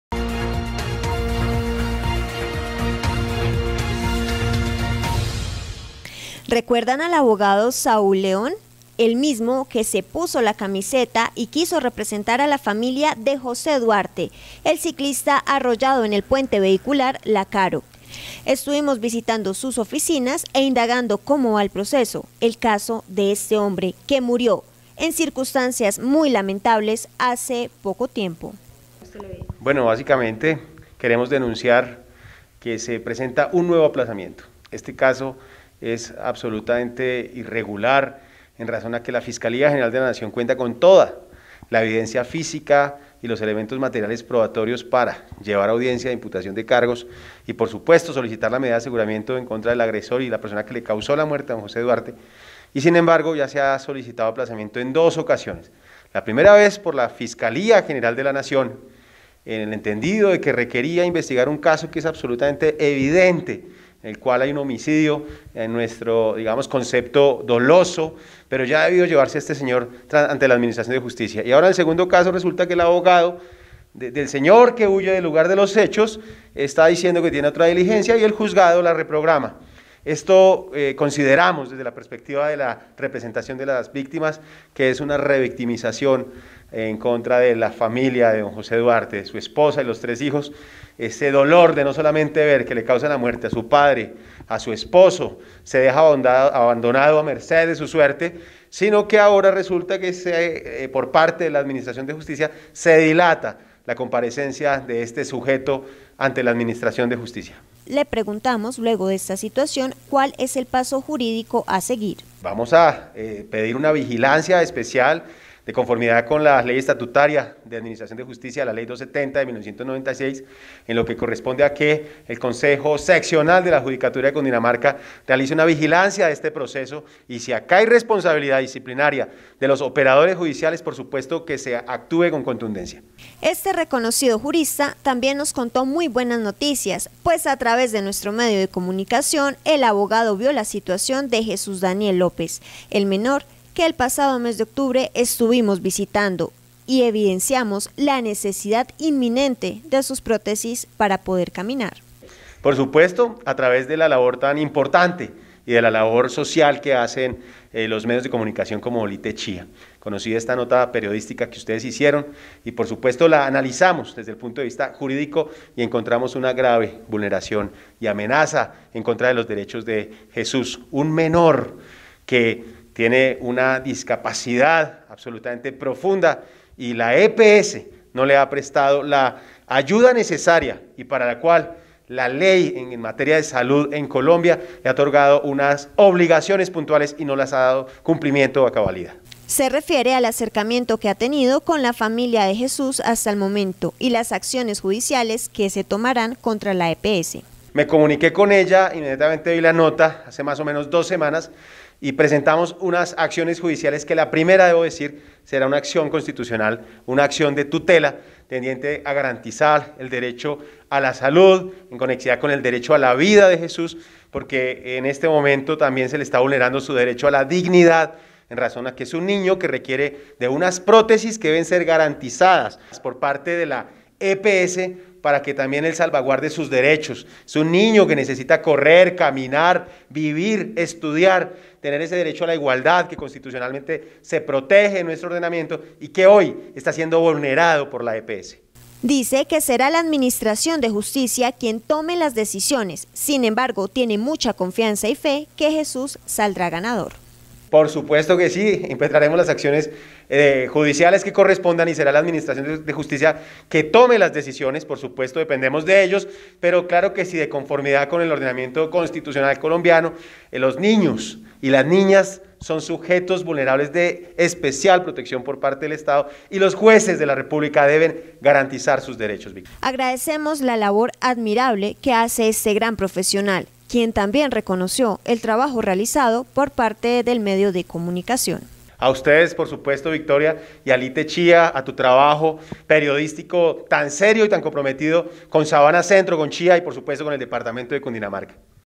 El Abogado Saúl León nos dio a conocer importantes noticias para los habitantes de Chía
Se refirió al avance en el proceso de José Duarte el Ciclista que fue arrollado en el Puente vehicular de la Caro, por otro lado le contó a LTC noticias que vio el informe de Jesús el niño que necesita sus prótesis para poder caminar y se pondrá en frente de este caso para velar por los derechos del menor.